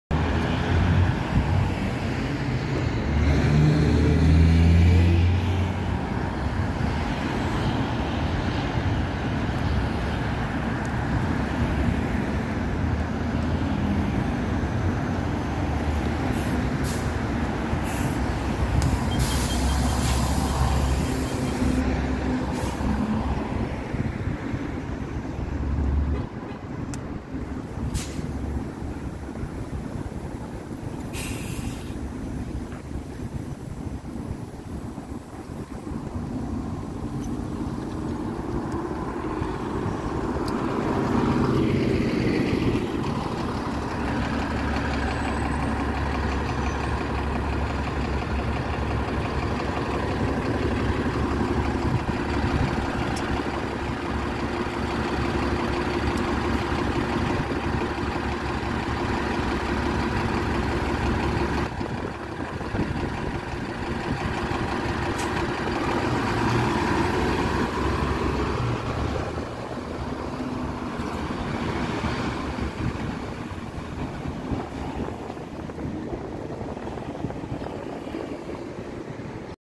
MTA New York City Bus: 2011 New Flyer XD40 [#4814] B45 at Atlantic & Vanderbilt Avenues
Stop Transfers:
B69: Kensington - Downtown Brooklyn